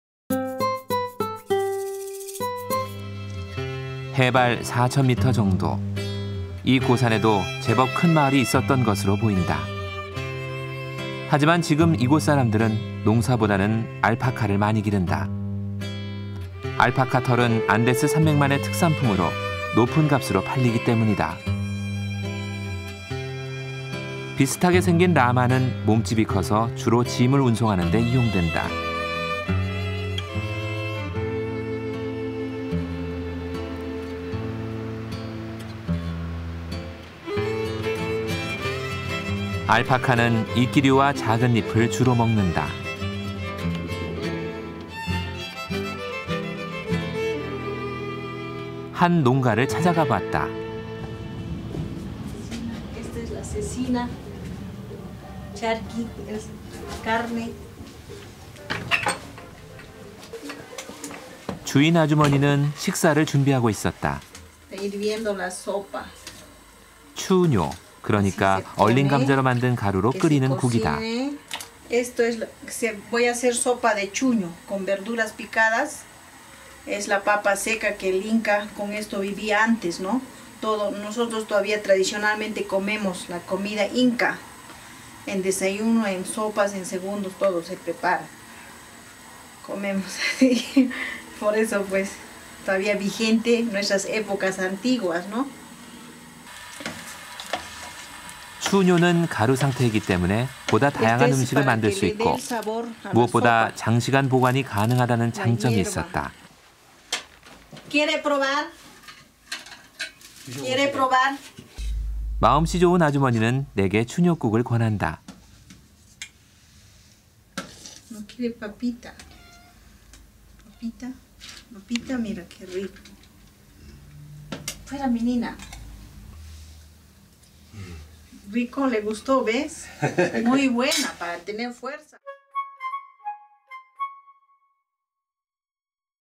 【K】Peru Travel-Cusco[페루 여행-쿠스코]잉카 전통 음식, 추뇨/Chuno/Inca/Food/Alpaca/Soup/Potato
[한국어 정보]
해발 4천 미터 정도 이 고산에도 제법 큰 마을이 있었던 것으로 보인다. 그러나 지금 이곳 사람들은 농사보다는 알파카를 많이 기른다. 알파카 털은 안데스 산맥만의 특산품으로 높은 값으로 팔리기 때문이다. 비슷하게 생긴 라마는 몸집이 커서주로 짐을 운송하는데 이용된다. 알파카는 이끼류와 작은 잎을 주로 먹는다. 한 농가를 찾아가 보았다. “알파카 고기예요.” 주인아주머니는 식사를 준비하고 있었다. 추뇨 그러니까 얼린 감자로 만든 가루로 끓이는 국이다. “이것은 ‘추뇨’라고 불리는 감자전분인데 잉카인들도 이걸 먹고 살았어요. 잉카의 전통음식이죠. 지금도 많이 만들어 먹어요. 주로 스프로 먹지만, 어떤 음식이든 만들 수 있어요.” 추뇨는 가루 상태이기 때문에 보다 다양한 음식을 만들 수 있고, 무엇보다 장시간 보관이 가능하다는 장점이 있었다. “드셔 보실래요? 감자도 좀 드릴까요?” 마음씨 좋은 아주머니는 내게 추뇨국을 권한다.

4000 m above sea level is also expected to Alpine was quite a big village. But now a lot of people here raise alpacas than farming. Alpaca wool is due to be sold only to a higher value to the specialty of the Andes. Similar-looking llama is used primarily to transport luggage petite cursor. Alpaca eats mainly small leaves and mosses. Visit saw a farmhouse. "It's alpaca meat." Landlady was preparing a meal. So chunyo a frozen soup made from boiled potatoes with flour. "It 'chunyo' inde Incas called the potato starch also lived to eat it. It's a traditional food of the Incas. Still I made a lot of eating. Mainly to eat soup, you can make some food or "chunyo can make a wider variety of food because powder state, what more can be stored for a long time had the advantage. "Want to try? Potatoes would get you? "Aunt recommend me a generous estimation nyoguk.

4000 m sobre el nivel del mar también se espera que los Alpes era un pueblo grande. Pero ahora un montón de gente aquí a recaudar alpacas que la agricultura. La lana de alpaca es debido a ser vendidos únicamente a un valor superior a la especialidad de los Andes. De aspecto similar de llama se utiliza principalmente para el transporte de cursor petite equipaje. Alpaca come hojas, principalmente pequeñas y musgos. Visita vio una casa de campo. "Es carne de alpaca." Casera estaba preparando una comida. Así chunyo una sopa congelada hecha de patatas cocidas con harina. "Es chunyo Inde Incas llamaban la fécula de patata también vivió a comer. Es un alimento tradicional de los Incas. Aún así que hice un montón de comer. Principalmente para comer sopa, usted puede hacer un poco de comida o" chunyo puede hacer una más amplia variedad de alimentos debido a estado de polvo, ¿qué más se puede almacenar por mucho tiempo tenía la ventaja. "¿Quieres probar? Patatas obtendrían usted?" La tía me recomiendan una generosa nyoguk estimación.

[Information]
■클립명: 남미015-페루05-09 잉카 전통 음식, 추뇨/Chuno/Inca/Food/Alpaca/Soup/Potato
■여행, 촬영, 편집, 원고: 서은섭 PD (travel, filming, editing, writing: KBS TV Producer)
■촬영일자: 2013년 7월 July

[Keywords]
,남미,America,아메리카,페루,Peru,Peru,,서은섭,2013,7월 July,쿠스코,Cusco,Cusco,Cuzco